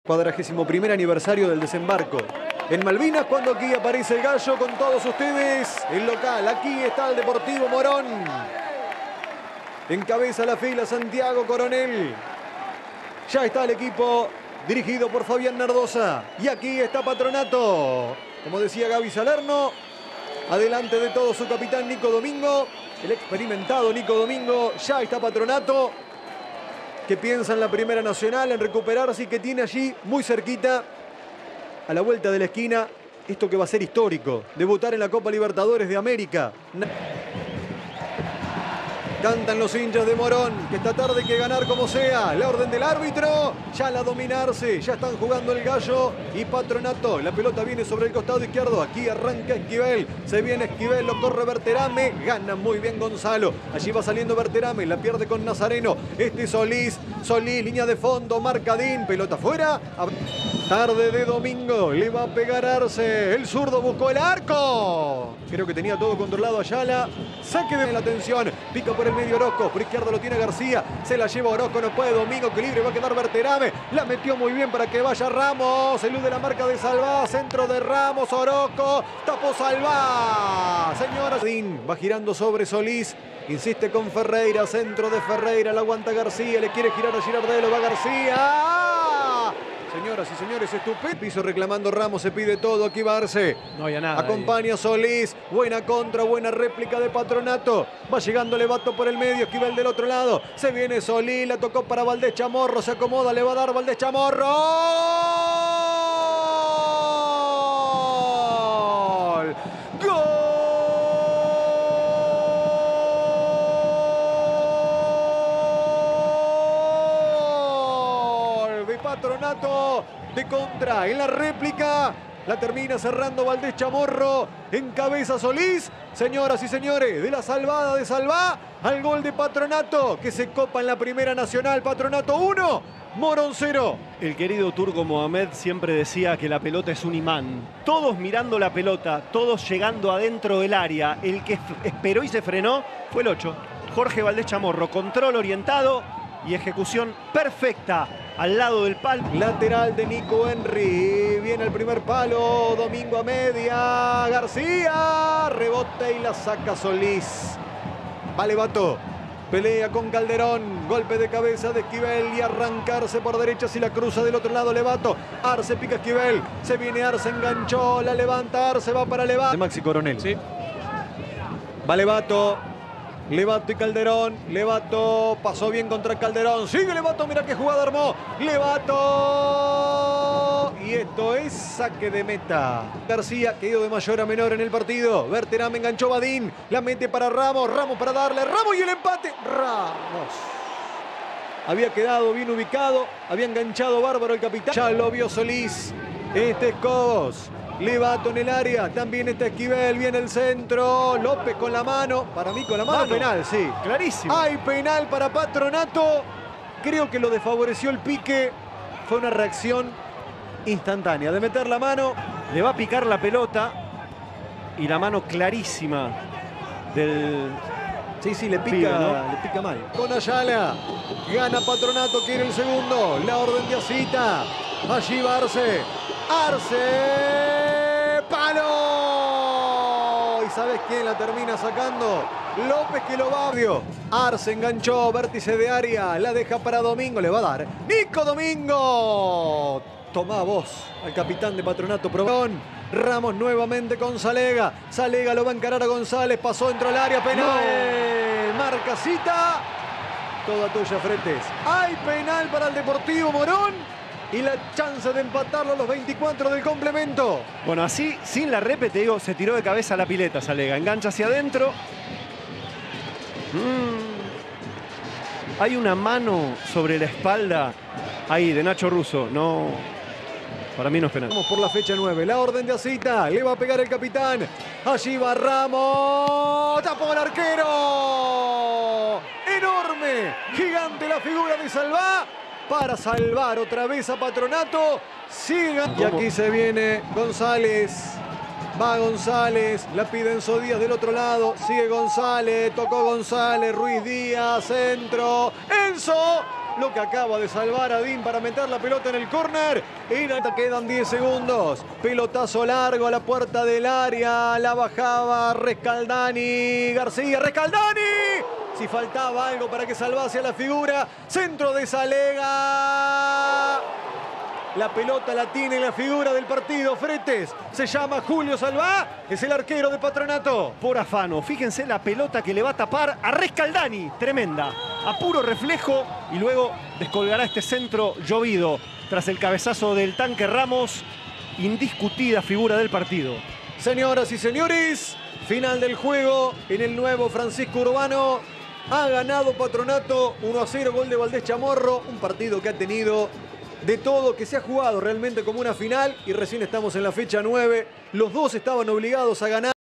Deportivo Morón 0-1 Patronato | Primera Nacional | Fecha 9 (Zona A)
Gol de Jorge Valdéz Chamorro a los 19´.